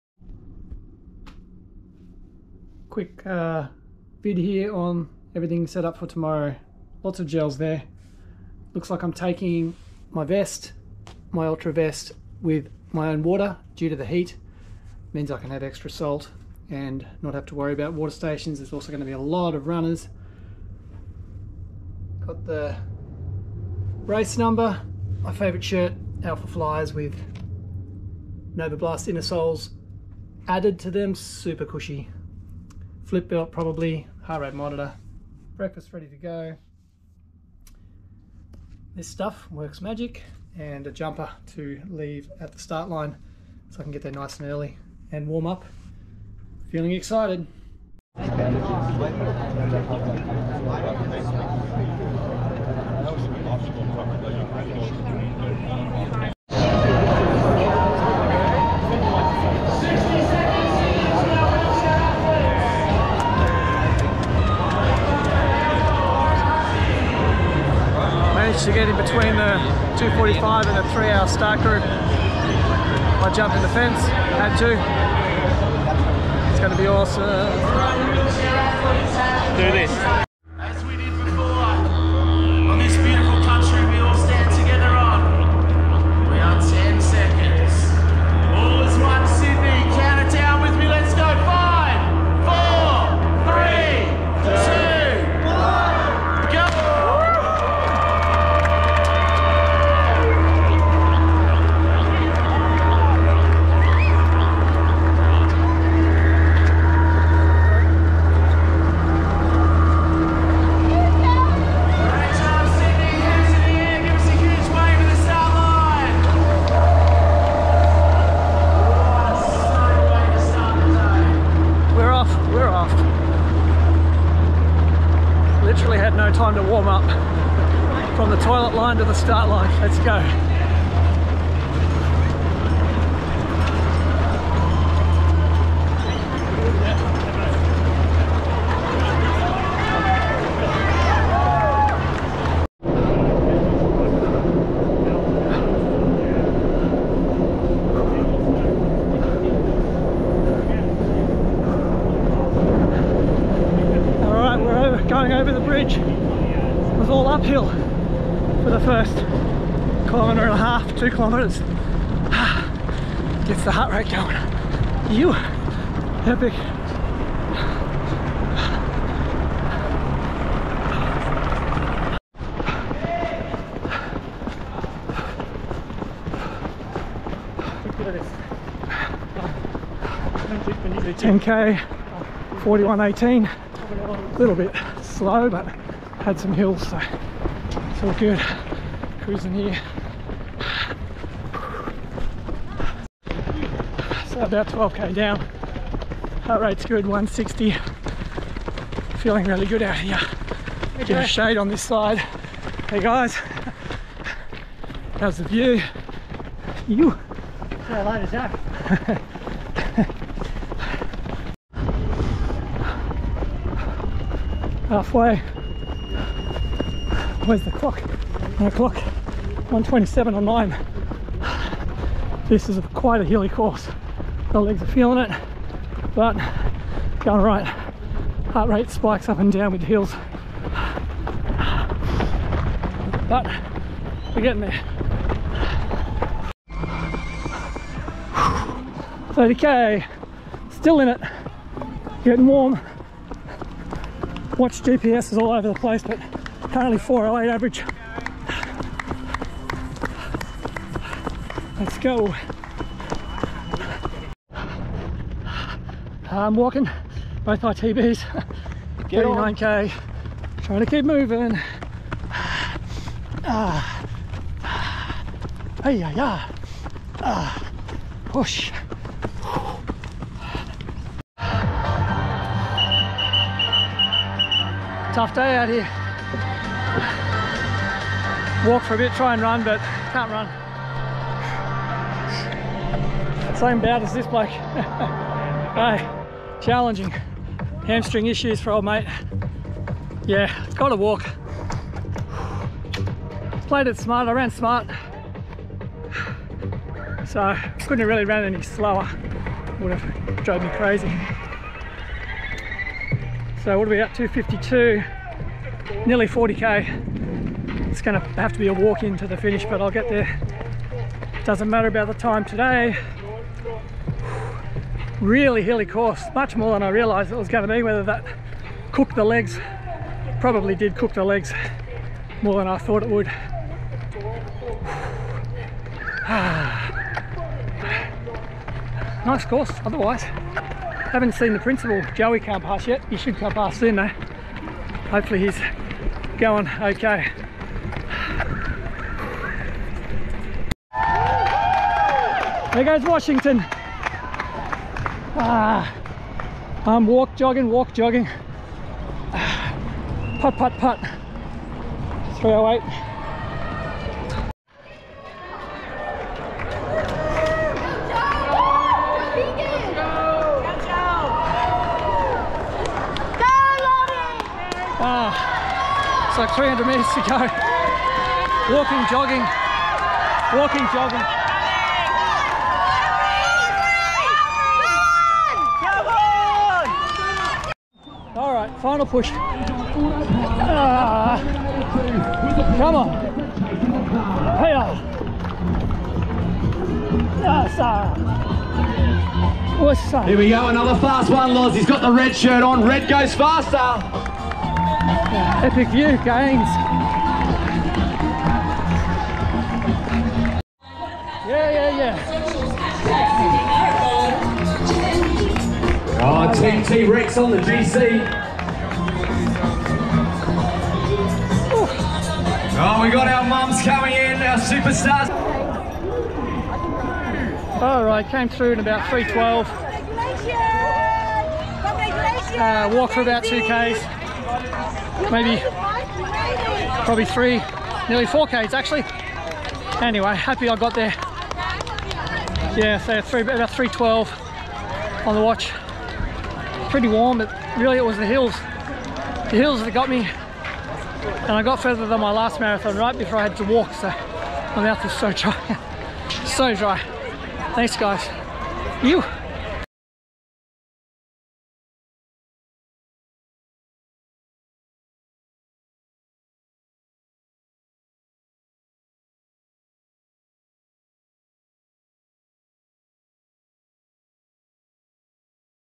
Sydney Marathon 2023 tough race!
Thanks for tuning in guys, was a tough day out with over 300m elevation gain and 30 degree celcius temperatures
So many competitors had rough days and so many run super strong, helps you appreciate fast flat courses like Gold Coast that's for sure!

Strava - Jo Tilbrook